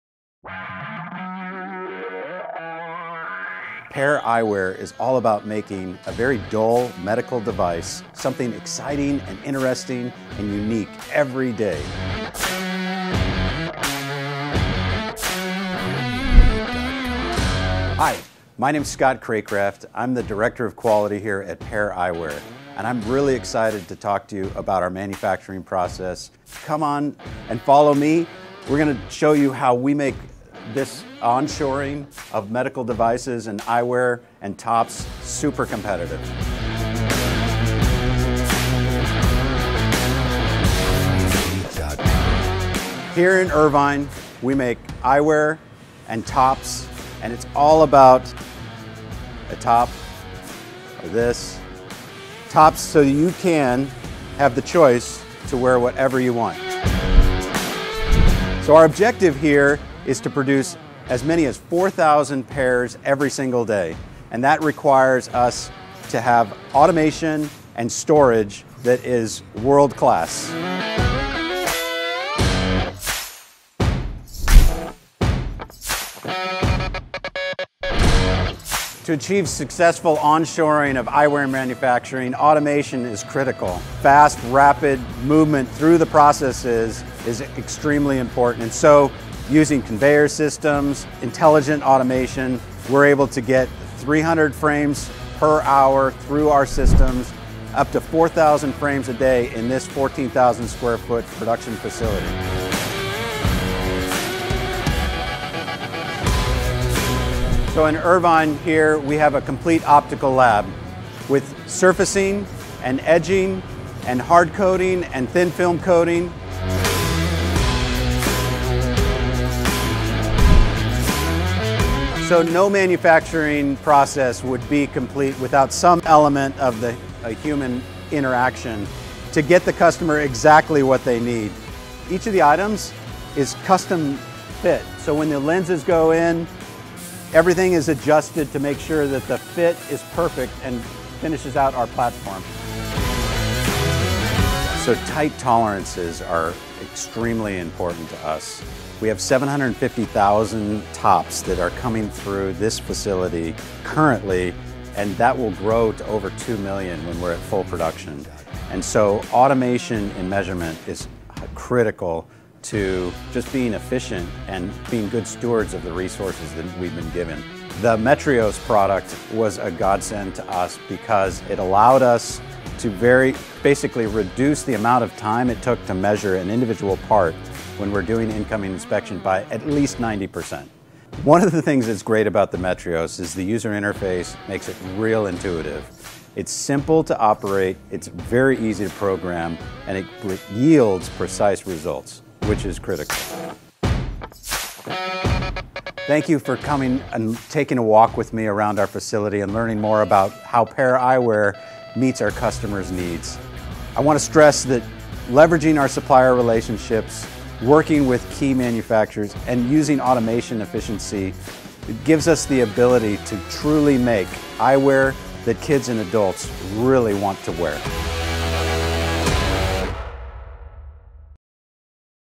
Pair Eyewear checks hundreds of frames with Metrios
So our objective here is to produce as many as 4000 pairs every single day and that requires us to have automation and storage that is world class.
To achieve successful on shoring of eyewear and manufacturing automation is critical.
Fast, rapid, movement through the processes is extremely important.
And so using conveyor systems, intelligent automation, we're able to get 300 frames per hour through our systems up to 4000 frames a day.

Each of the items is custom fit, so when the lenses go in, everything is adjusted to make sure that the fit is perfect and finishes out our platform.
So tight tolerances are extremely important to us.

The Metrios product was a godsend to us because it allowed us to very basically reduce the amount of time it took to measure an individual part when we're doing incoming inspection by at least 90%.

One of the things that is great about the Metrios is that the user interface makes it real intuitive, it's simple to operate, it's very easy to program, and it yields precise results, which is critical.